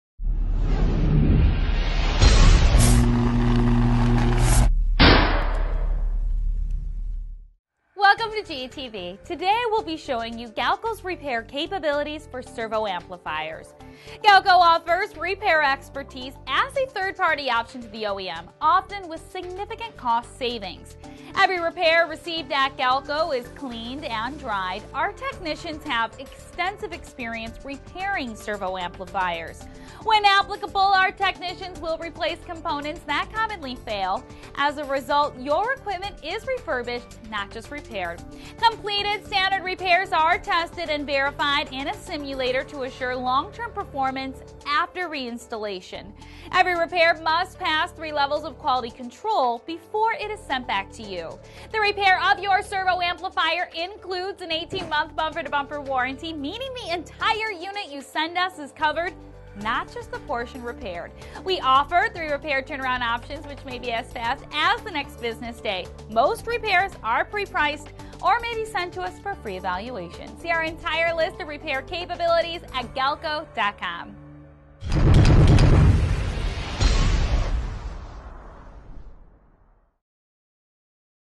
Servo Amplifier Repair
Servo Amplifier repaired by experienced technicians.  Katie Rydzewski presents Galco's repair capabilities.  to get your item repaired.

18-Month 'Bumper-to-Bumper' Warranty -- The entire unit you send to us for service is covered under warranty, not just the portion repaired.